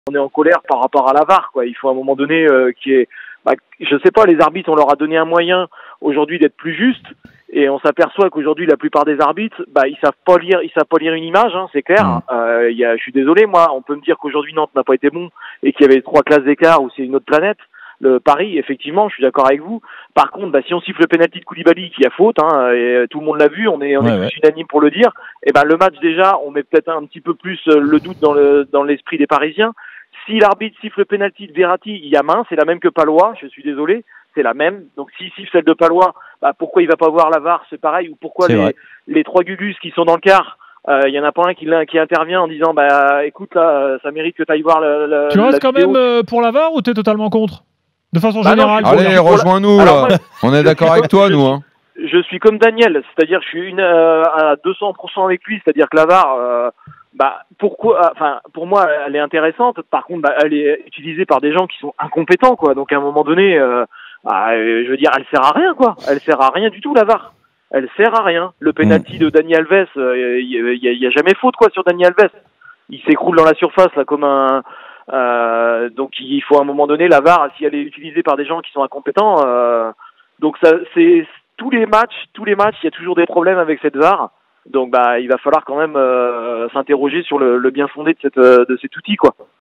Le coup de gueule d’un supporter nantais contre le VAR
Après l’élimination de Nantes en demi-finale de Coupe de France face au PSG (3-0), l’un des supporters des Canaris n’a pas caché sa colère contre le VAR et sa mauvaise utilisation par les arbitres.